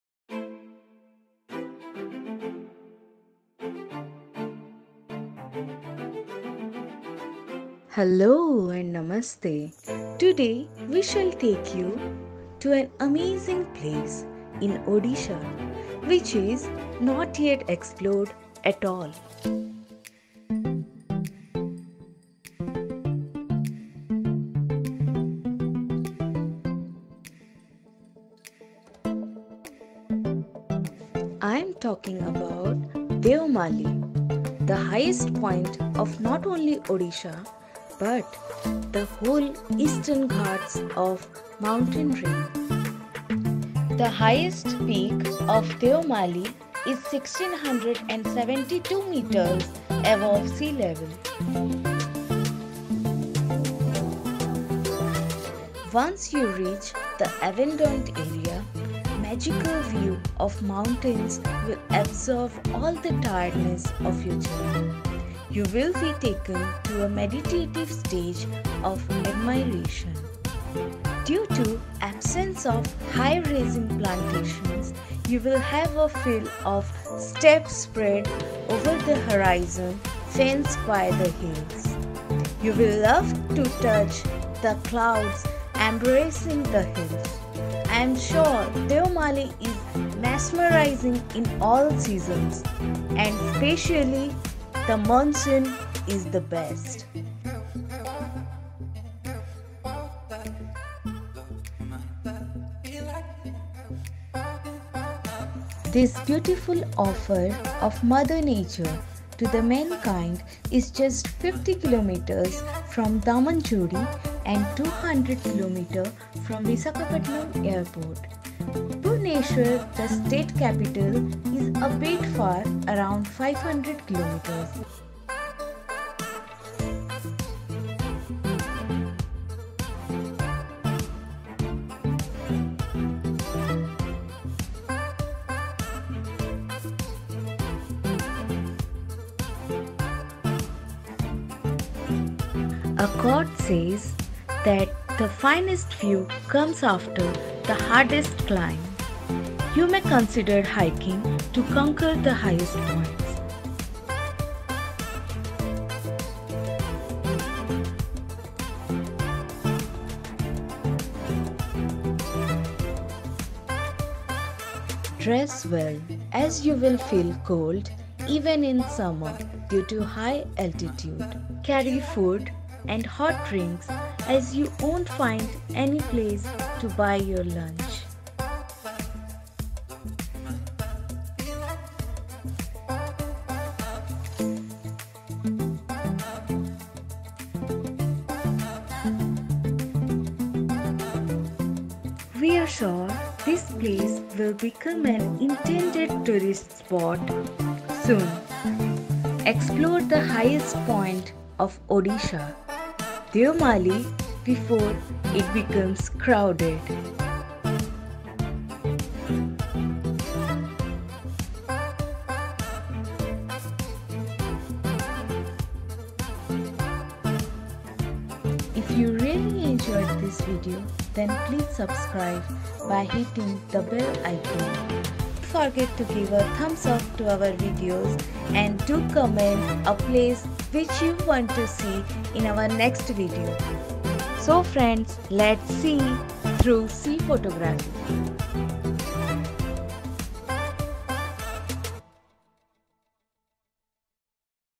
Deomali ,The heightest peak of Odisha
Hello, Namaste, Juhar.
Today we shall take you to an amazing place in Odisha which is not yet explored at all. I am talking about D E O M A L I, the highest point of not only Odisha but the whole eastern ghat mountain range. The highest peak of Deomali is 1672 meters above sea level.
This beautiful offer of Mother Nature to the mankind is just 50km from Damanjodi and 200 km from Vishakhapatnam airport. Bhubaneshwar, the state capital is bit far, around 500 km.
Once you reach the abandoned area, magical view of mountains will absorb all the tiredness of your journey.  You will be taken to a meditative stage of admiration. Due to absence of high rising plantations you will have a feel of steppe spread over to the horizon fenced by the hills. You will love to touch the clouds embracing the hills. I am sure that Deomali is mesmerizing in all seasons except monsoon.
Dress well as you will feel cold even in summer due to high altitude. A quote says that the finest view comes after the hardest climb. You may consider hiking to conquer the highest point.  Carry food and hot drink as you won’t find any place to buy your lunch.
We are sure this place will become an intended tourist spot soon. Explore the highest point of Odisa – Deomali before it becomes crowded.